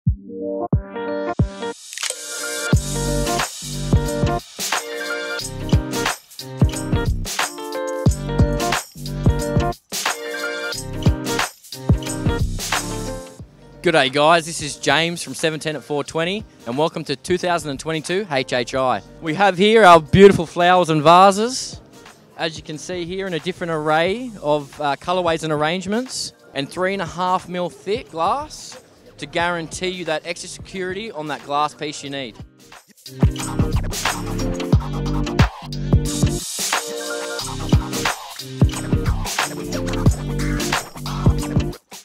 710@420 Glass Blowing at @HhiexpoAu Sydney Olympic Park
710@420 booth at Hemp, Health & Innovation Expo Olympic Park 2022.

Video of @empirical_glass whipping up a Sherlock by @blvckoutmedia out now.